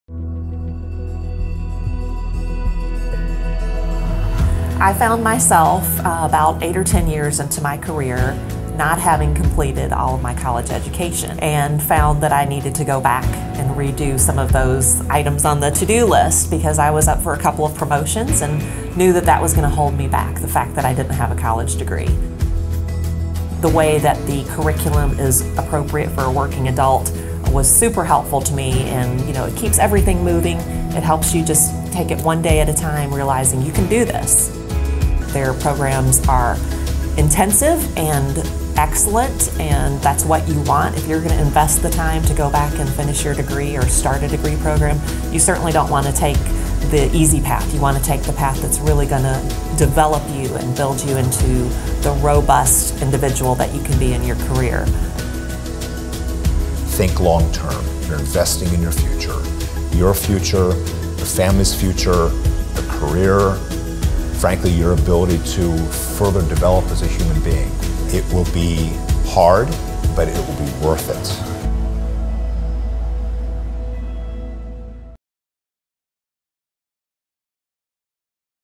Meet Robin | Adult and Graduate Studies
Robin Pugh, President of Queen Associates, explains how the intensive and excellent curriculum at Montreat’s School of Adult and Graduate Studies is helpful to working adults looking to complete a degree.

To learn more about our online programs or to apply today,